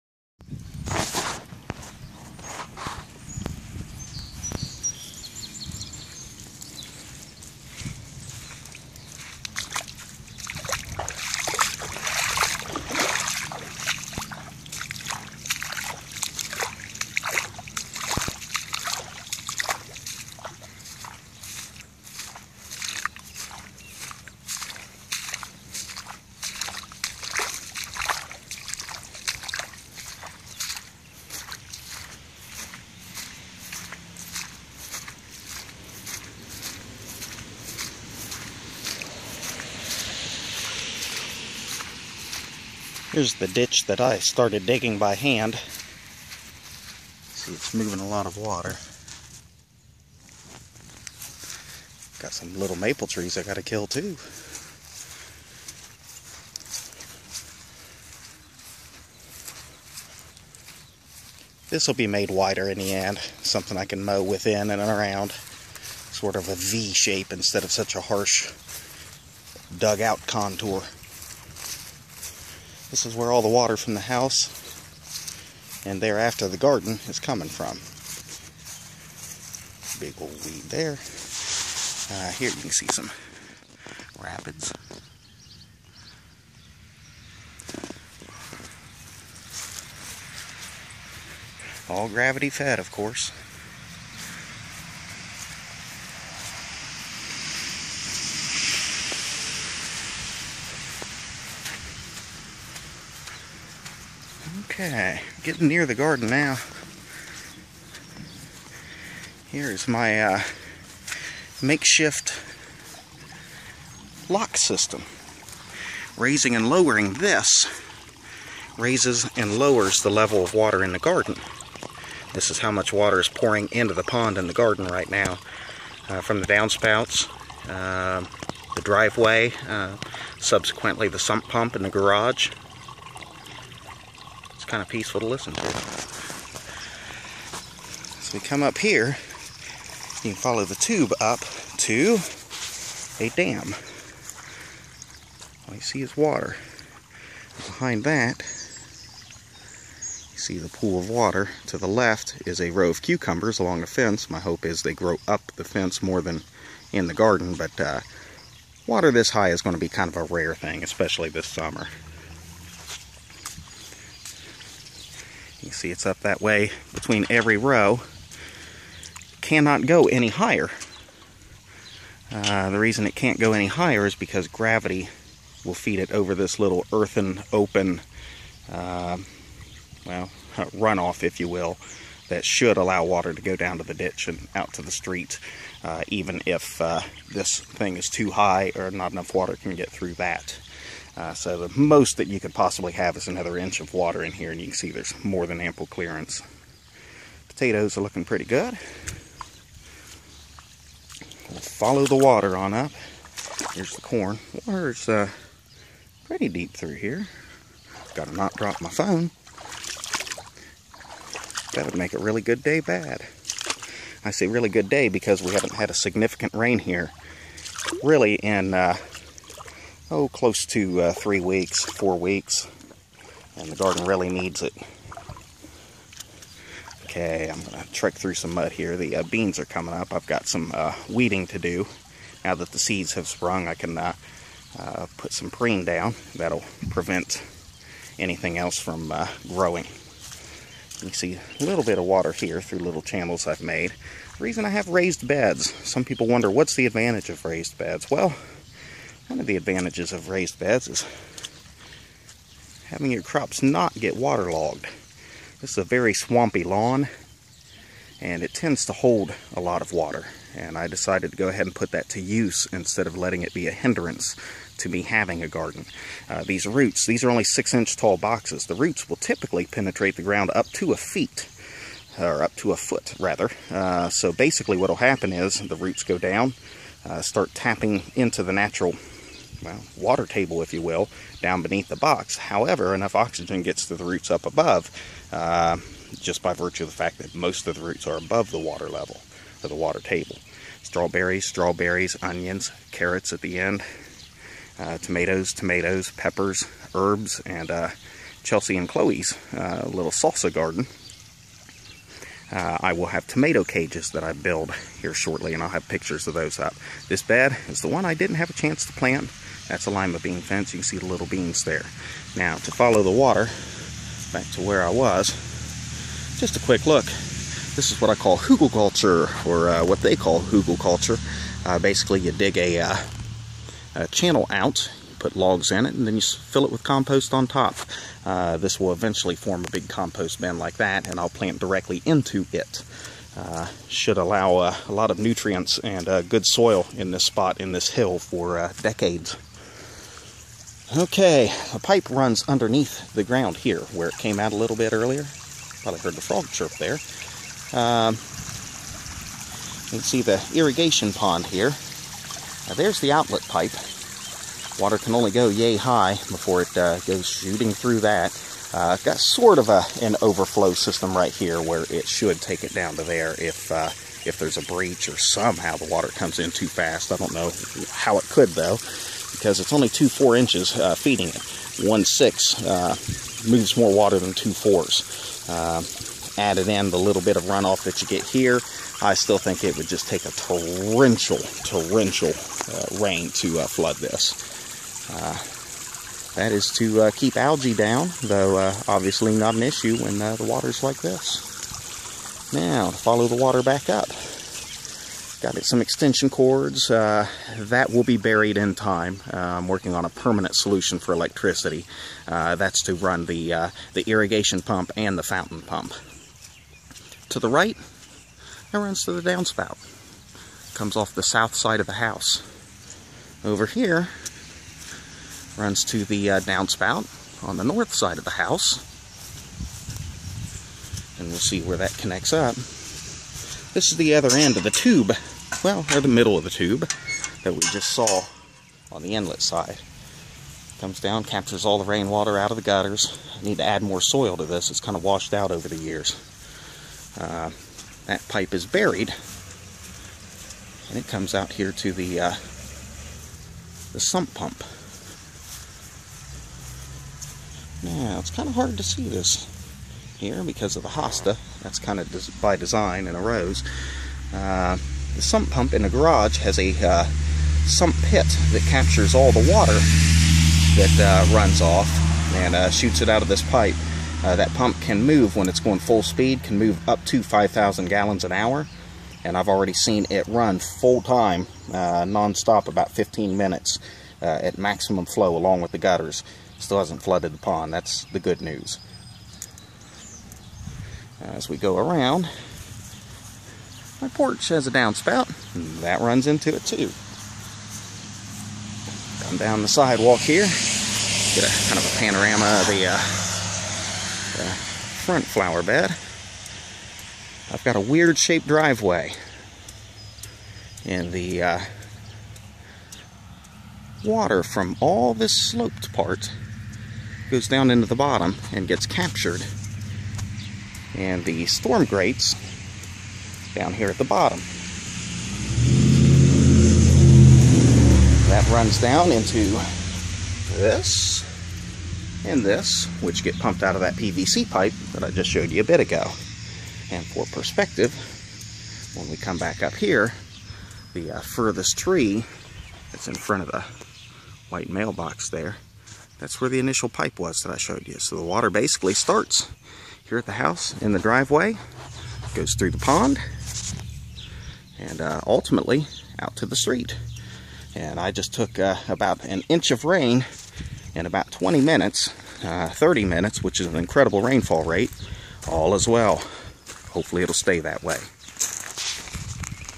Spring 2014 Garden irrigation walk through.
I do a quick walk-through of my (still unfinished) garden to show how I am using runoff and gravity to my advantage for water storage and irrigation.  Those of you who have wanted to see how it all works will get a glimpse.  ...I even casually narrate the thing after starting at the street where all of the extra water eventually goes.  Please ask any questions.  Note, in Ohio, not only is rain water harvesting legal, it is encouraged for crop irrigation.  This may not be the case where you live.